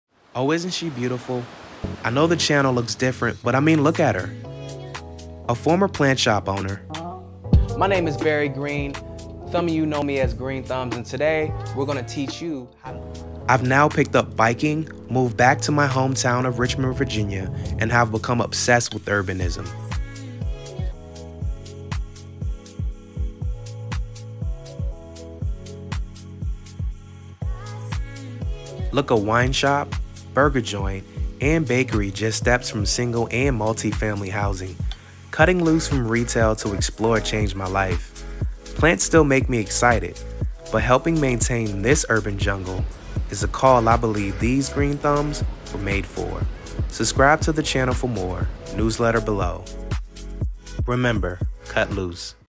Moving Back to Richmond | Exploring Richmond, VA
Are you a fan of urbanism and/or minimalism?

I stepped away from owning a plant shop to explore, re-prioritize what’s important and study new interests.

This trailer gives you a preview of what to expect from my channel.

Featuring a snippet shot by Rob Journey

Musician: Jef